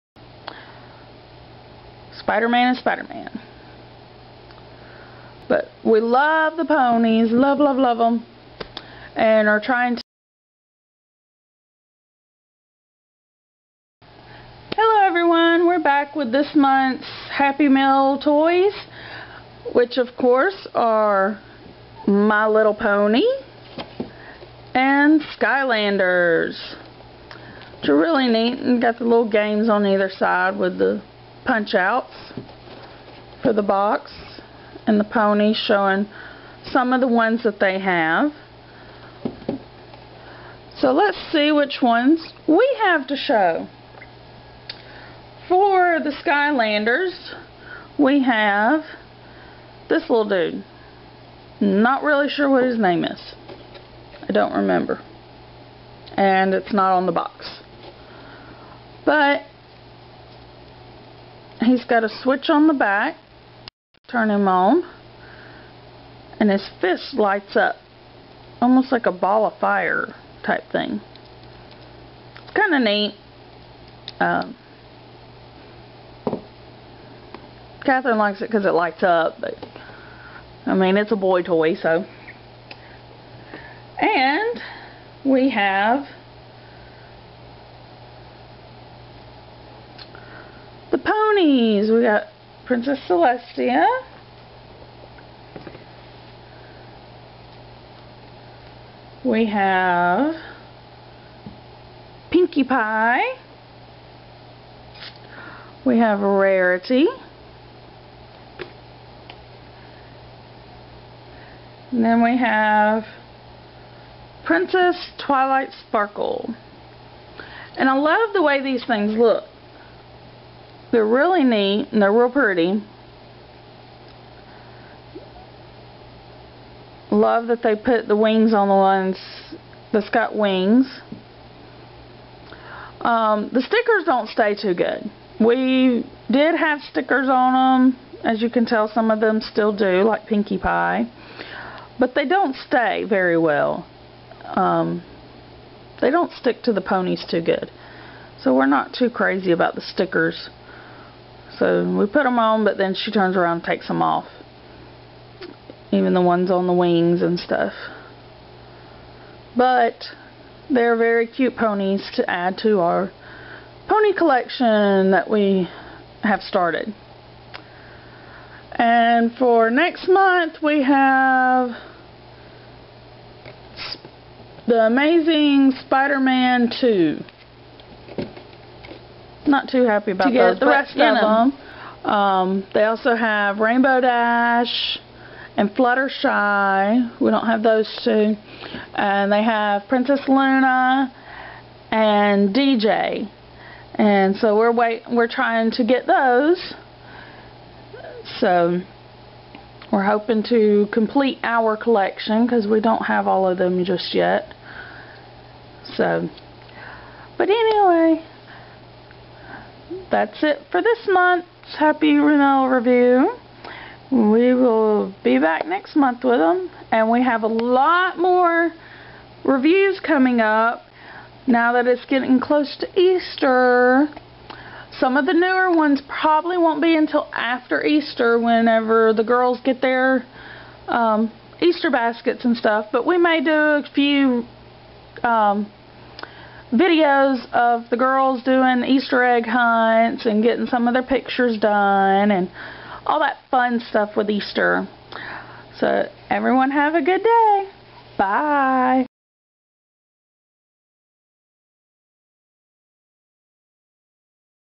My Little Pony Happy Meal Toys Part 1
Monica reviews 4 of the MLP happy meal toys

CCMreviews
Maylene, AL 35114